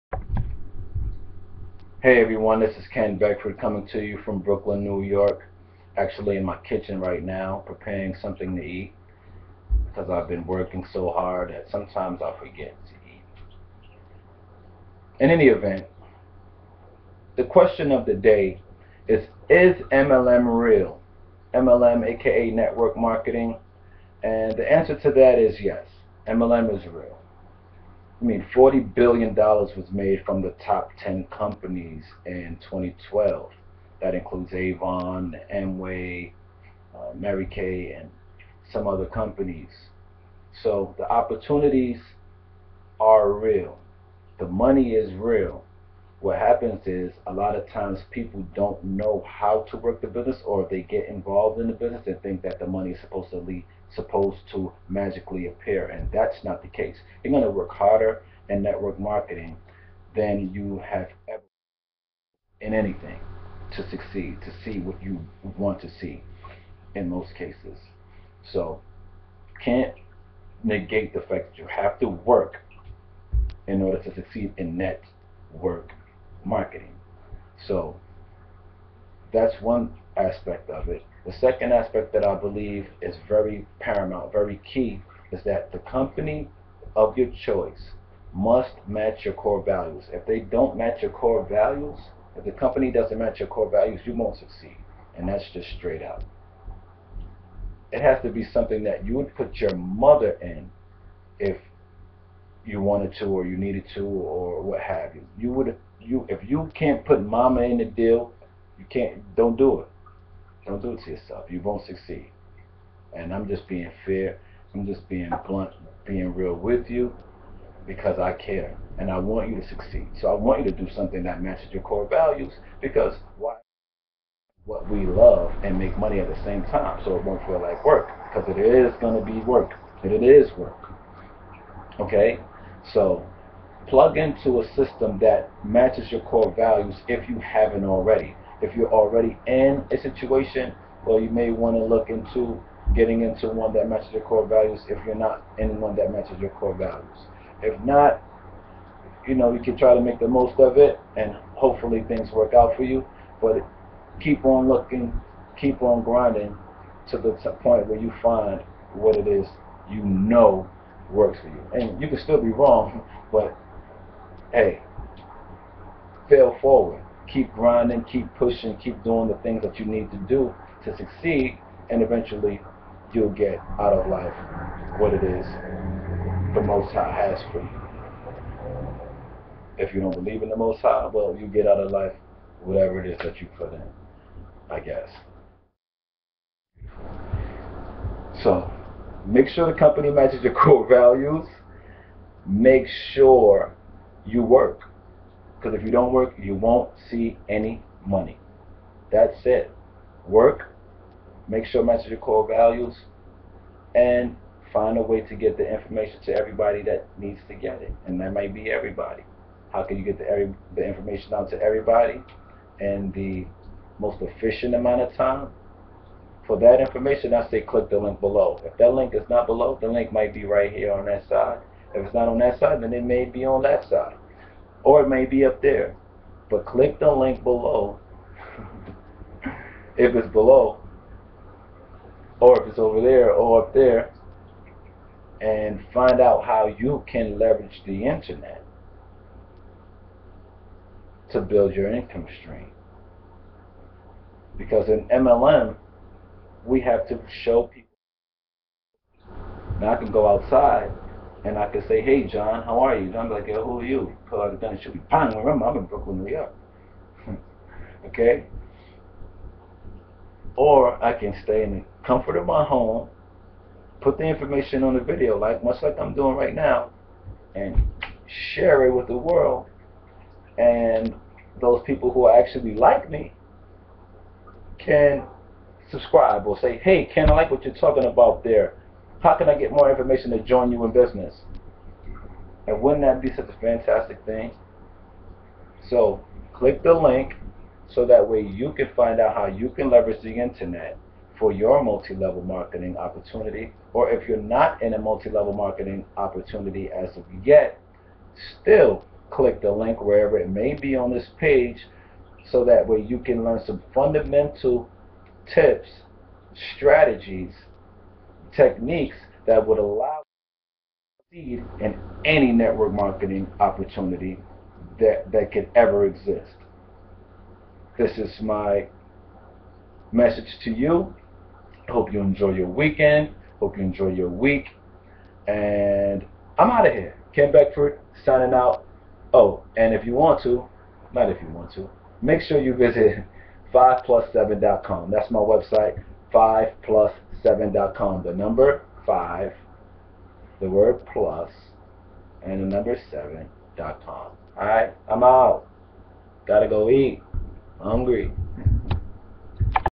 Does MLM Really Work?
A brief answer to the question, "Does MLM Really Work",
by Ken Beckford,
Online Marketing Trainer